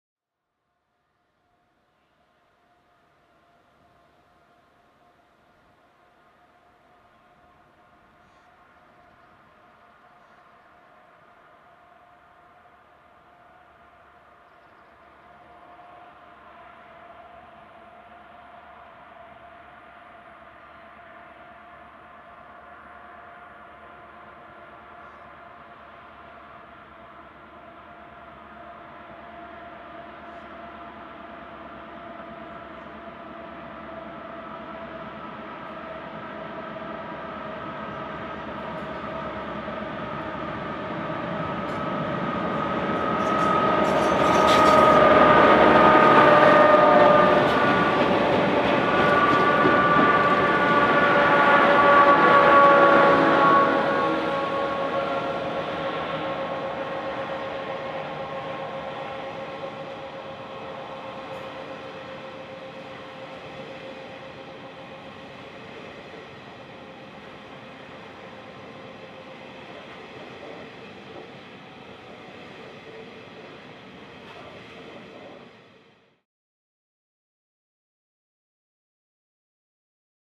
S303-T357-T333-T378-B74 Ravenswood Tue 04/01/11
S303-T357-T333-T378-B74 roll down from Big Hill tunnel as they approach Ravenswood loop with ELZ's 9062 grain from Woorinen.  Tue 04/01/11